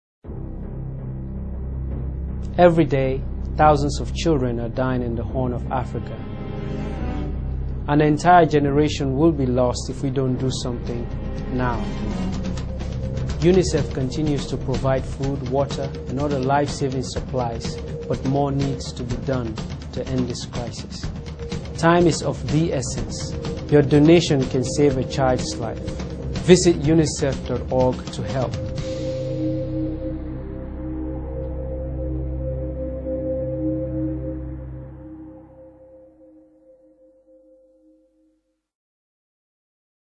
Ishmael Beah PSA addressing the crisis in the Horn of Africa
UNICEF YouTube-5 October 2011- UNICEF Goodwill Ambassador, Ishmael Beah, makes an appeal for continued support in the Horn of Africa